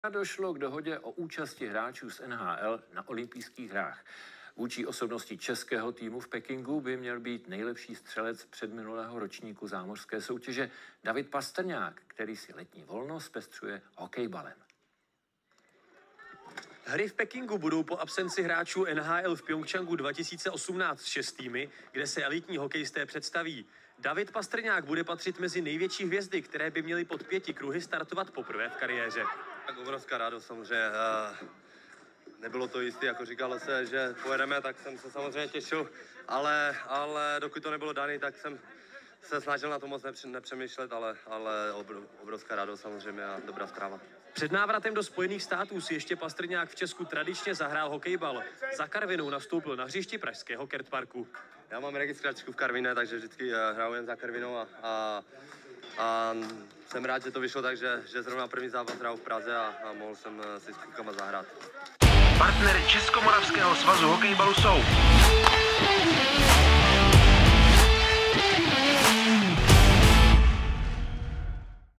Reportáž: Hvězda NHL David Pastrňák si zahrál hokejbal za Karvinou!
Zpestřil si přípravu a oprášil registračku. Hokejový kanonýr David Pastrňák se 1. kole nové sezóny Extraligy hokejbalu představil v dresu hokejbalistů Karviné na hřišti Kert Parku Praha. Hvězda Bostonu a nejlepší střelec NHL ze sezóny 2019/20 se na hřišti vítěze čtyř posledních dohraných ročníků při porážce 2:5 zapsal i mezi střelce svým typickým přesilovkovým zásahem.